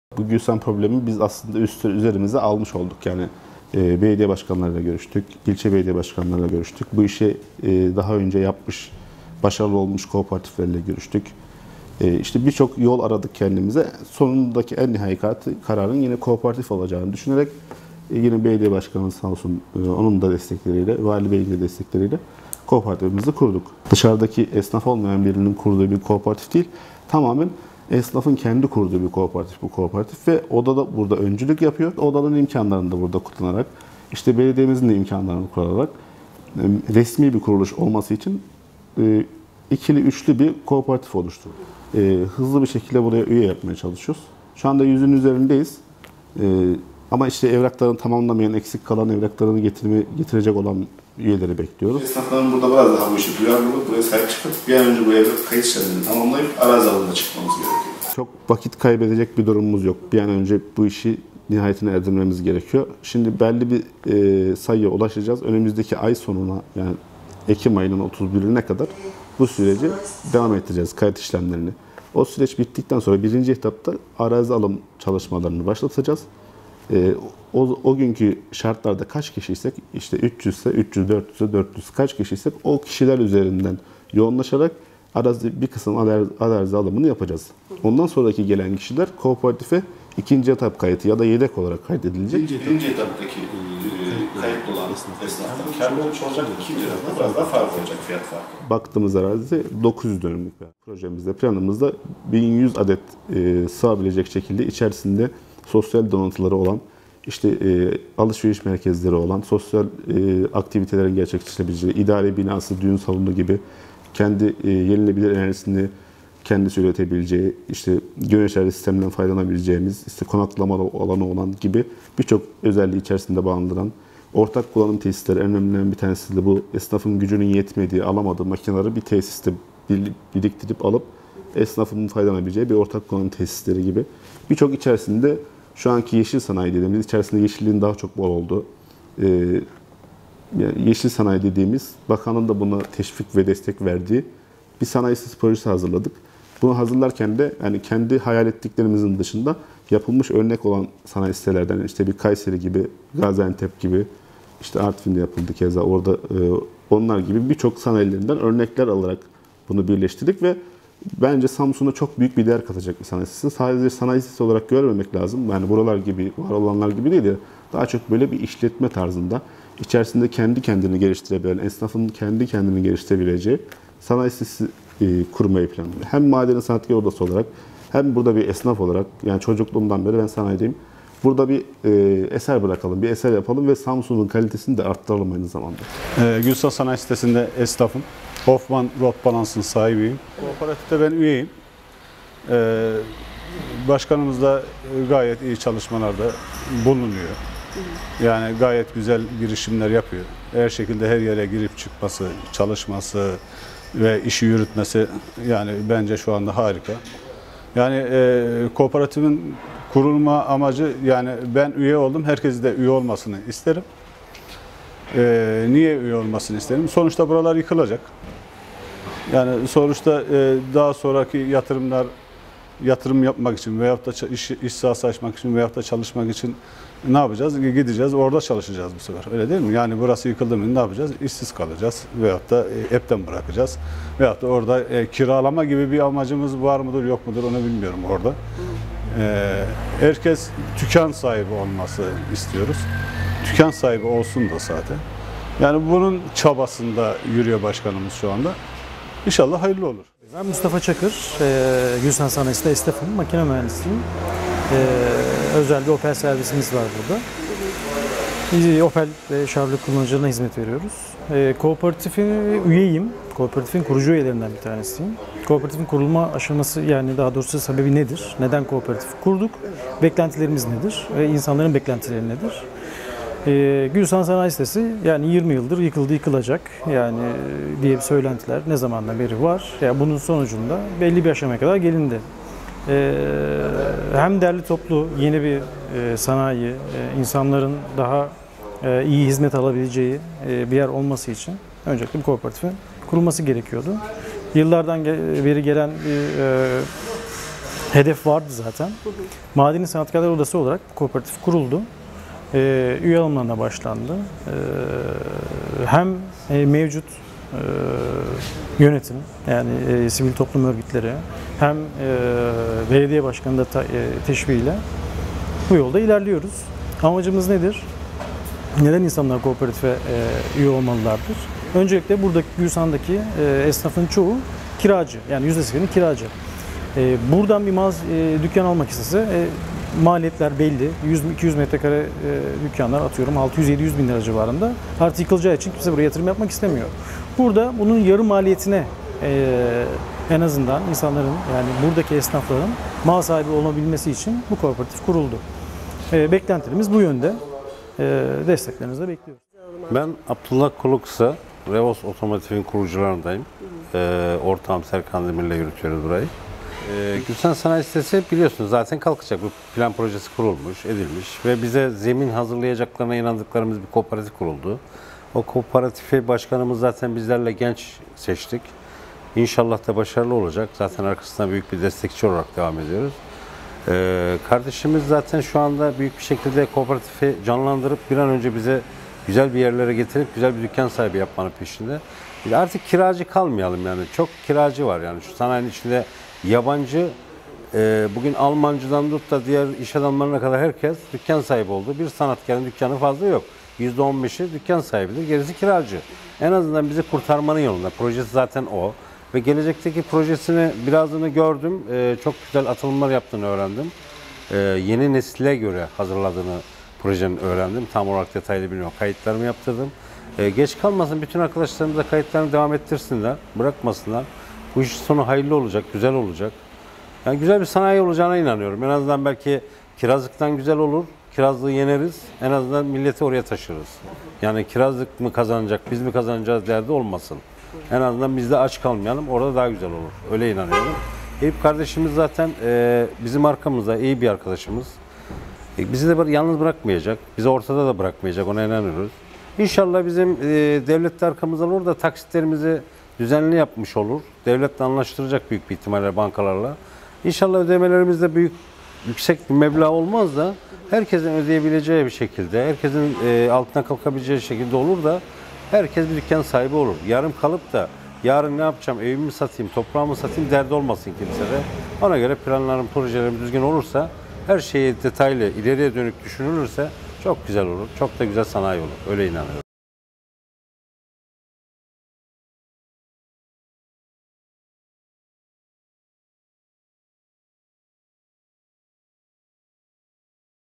Aydın’dan esnafa çağrı| Samsun ve Haber
Samsun Madeni Sanatkarlar Odası Başkanı Eyüp Aydın, Gülsan Sanayi esnafının kooperatife sahip çıkması gerektiğini belirtti. | Samsun ve Haber